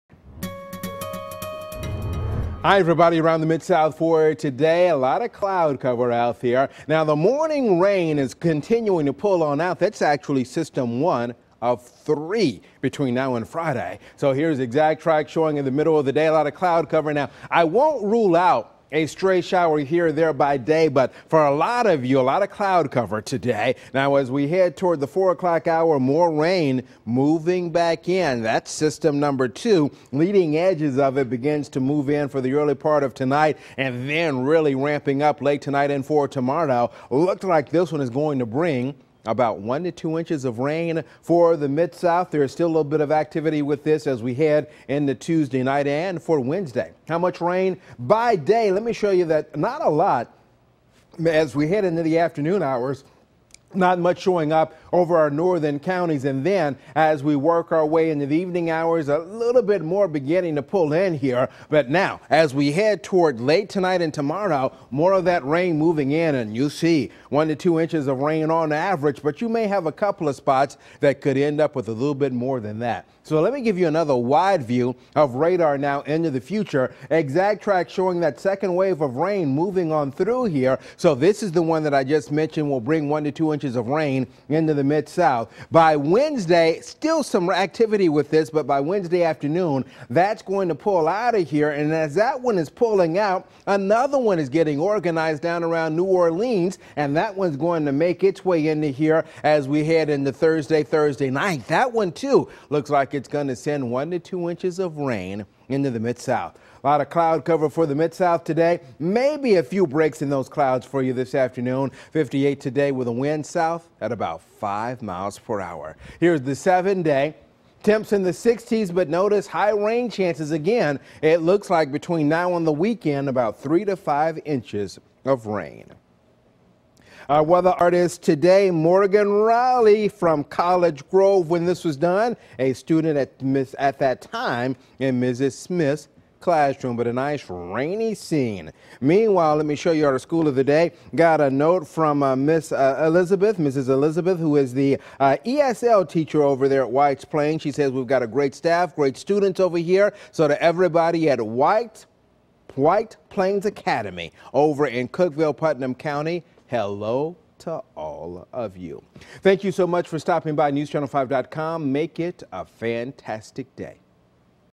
Lelan's Morning Forecast: Monday, March 9, 2015
Lelan Statom's morning weather forecast for Monday, March 9, 2015.